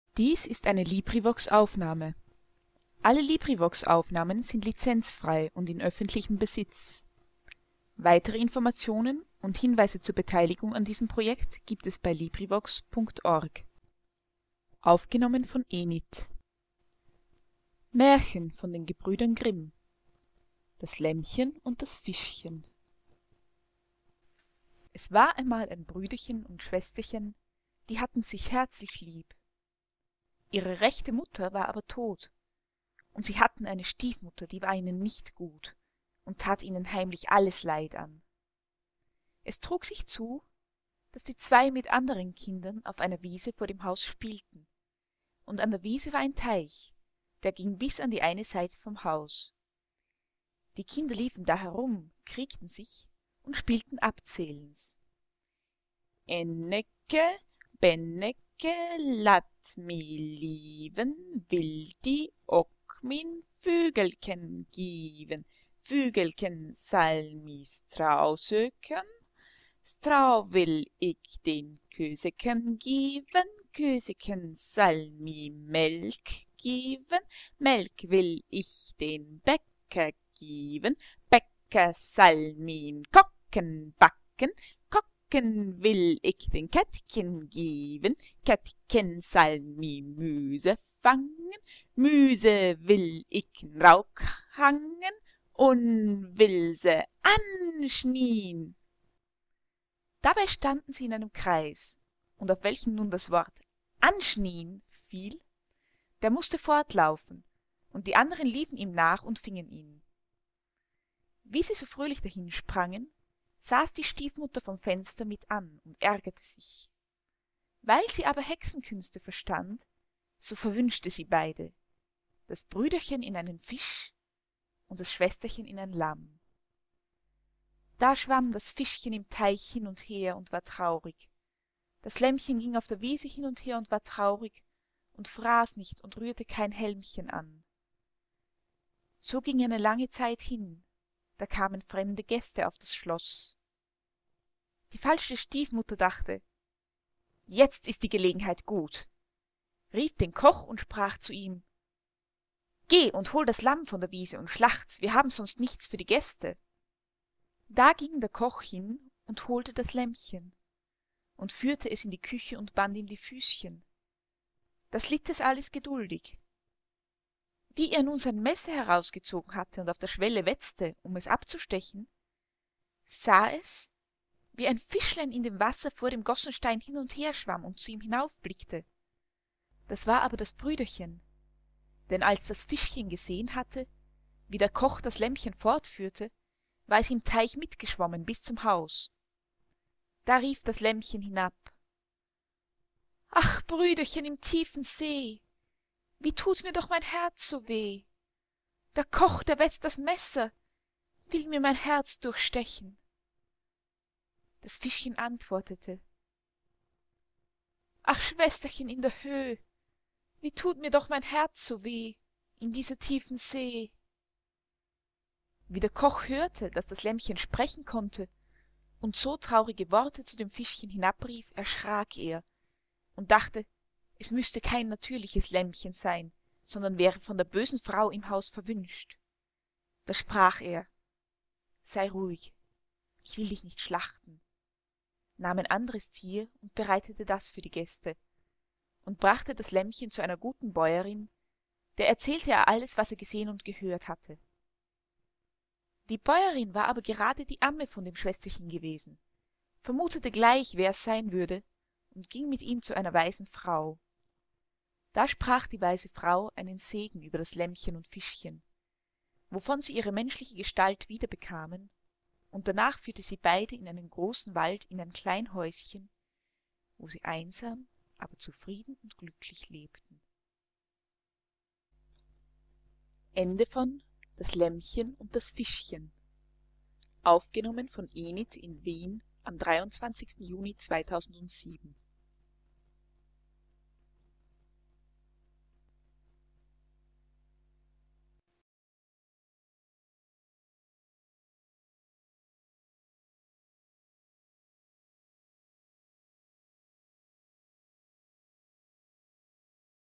011 das laemmchen und das fischchen Gebrüder Grimm
Librivox recording of Maerchen, von den Gebruedern Grimm.

Anfang des 19. Jahrhunderts begannen die Brueder Jacob und Wilhelm Grimm, traditionelle, bisher vor allem muendlich weitergegebene Erzaehlungen zusammenzutragen. Diese Sammlung ist als die Maerchen der Gebrueder Grimm weltbekannt geworden und umfaßt so beruehmte Geschichten wie Rapunzel, Haensel und Gretel, Rumpelstilzchen, Rotkaeppchen und viele andere. Diese Maerchen wurden spaeter stark editiert und 'beschoent'. Die hier vorliegenden Versionen entsprechen aber staerker den originalen, teils drastischen Fassungen.